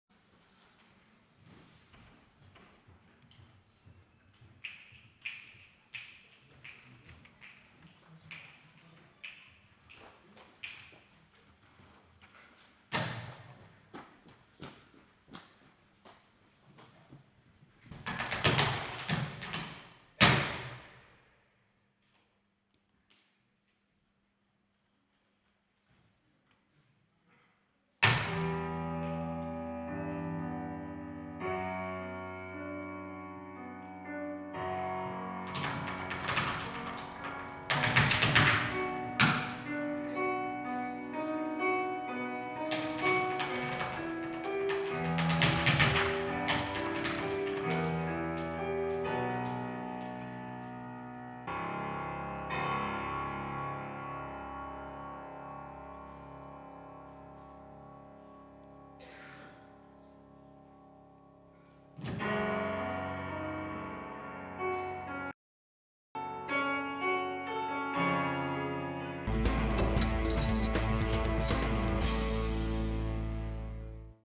Iván Góngora i Yaiza de los Muros protagonitzen 'El Novio' a l'Auditori Municipal de Terrassa
És una obra de pur flamenc, en la que la passió i la intensitat són els protagonistes. Aquí en teniu una mostra.